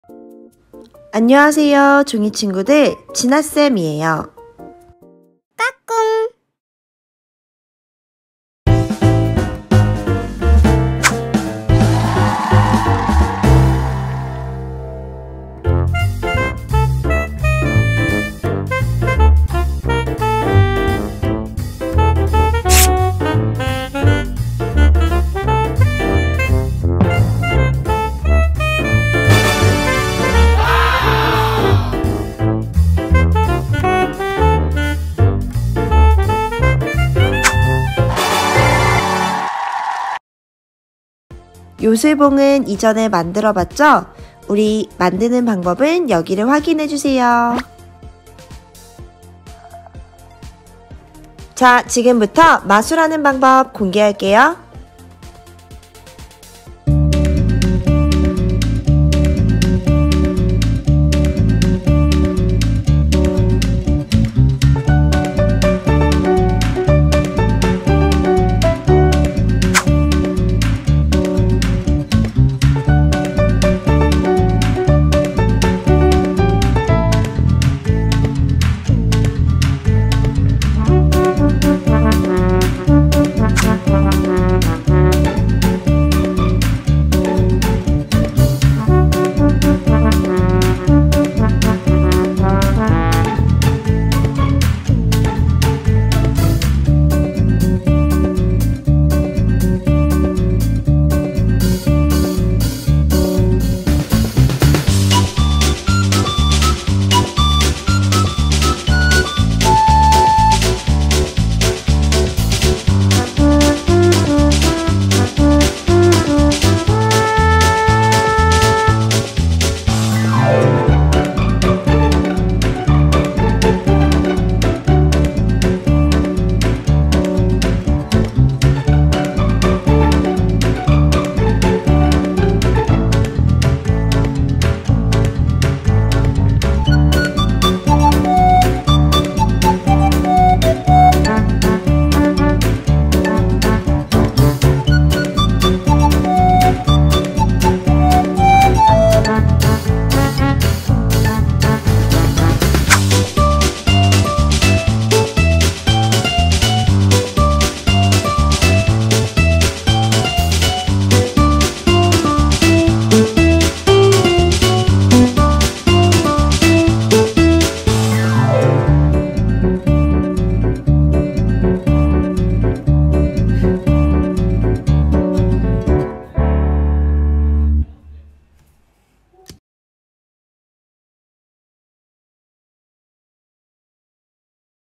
[종이접기] 순간 이동하는 요술봉 ?! / 마술 공개 / magic stick
00:00 인트로
00:08 마술 시작
00:41 요술봉 만드는 영상 소개
00:53 마술 하는 방법 공개